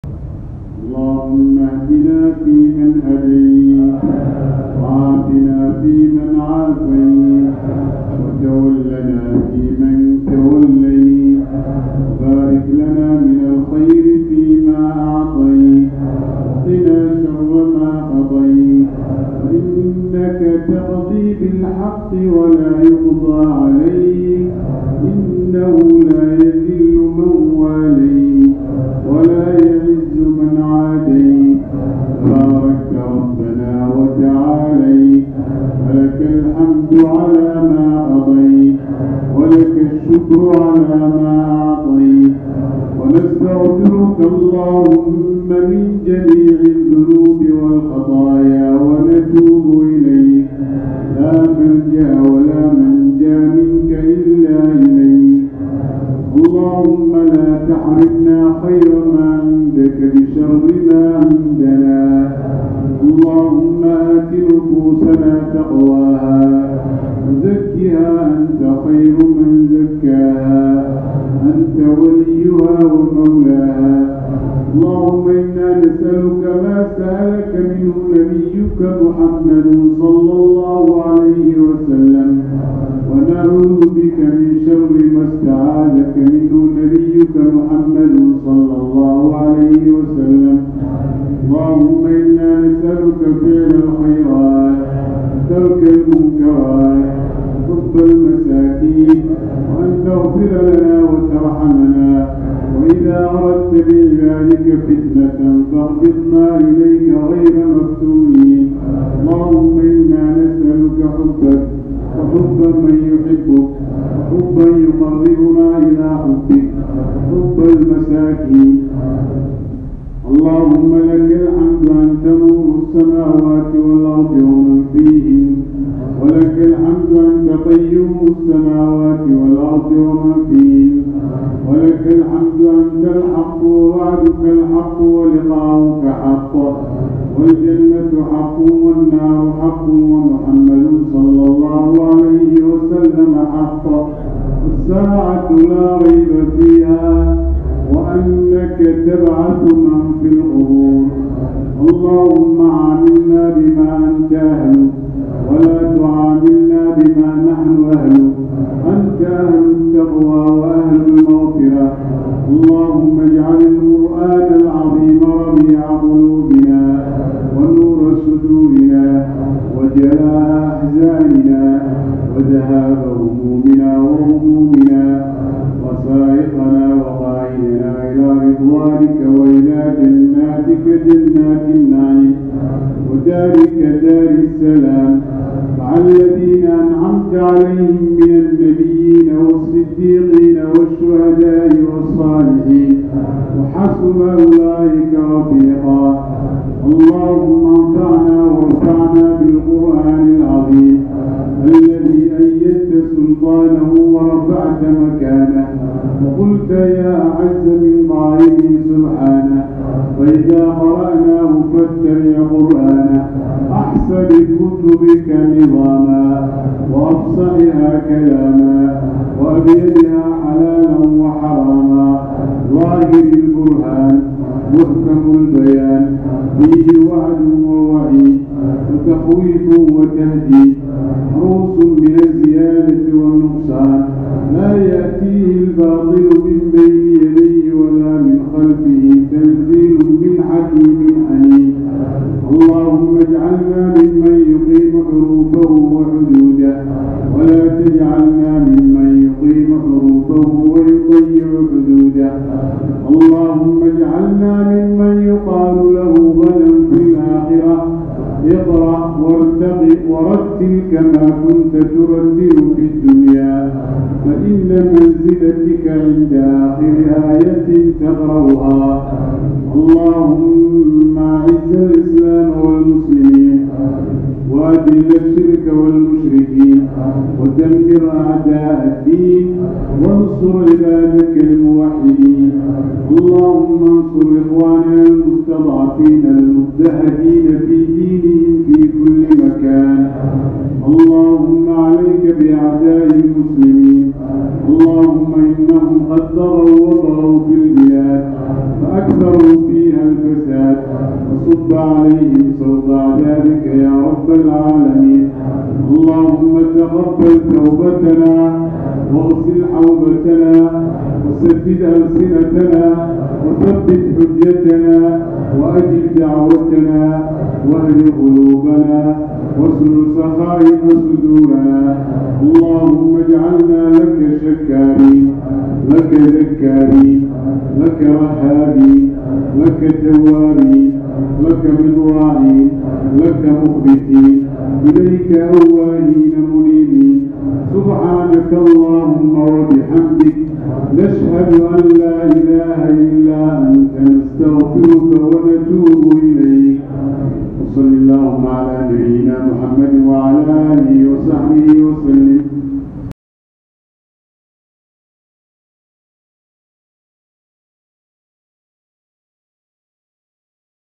Ducadii Salaada Taraawiixda ee Masaajidka Tawfiiq Sheekh Cilmi Maxamed | Xaafada Siinay | Dawsho Wcn
Sheekh Cilmi Maxamed iyo Ducadii Salaada Taraawixda Habeenkii 11 Ramadaan 1444.